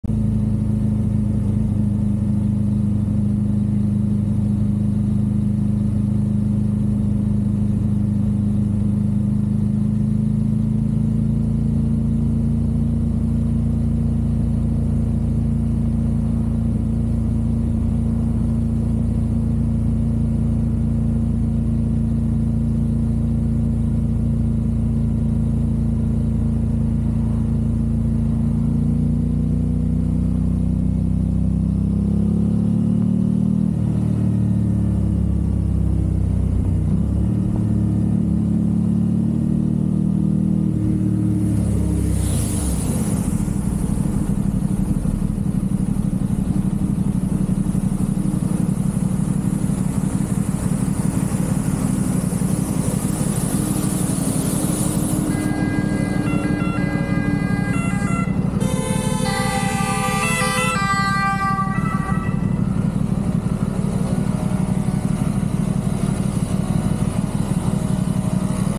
Verrückte Frau rennt vor einen Krankenwage | RS660 + Mivv Delta Race | Pure sound
Entscheide mit der Community wie wir das bike tunen sollen:
BIke:  @ApriliaOfficial  RS660
Filmed with  @insta360  X3 Camera.
Exhaust  @MivvExhausts  Delta Race (low mounted).

Chapters:
Chill ride(0:00)
Crazy lady(0:51)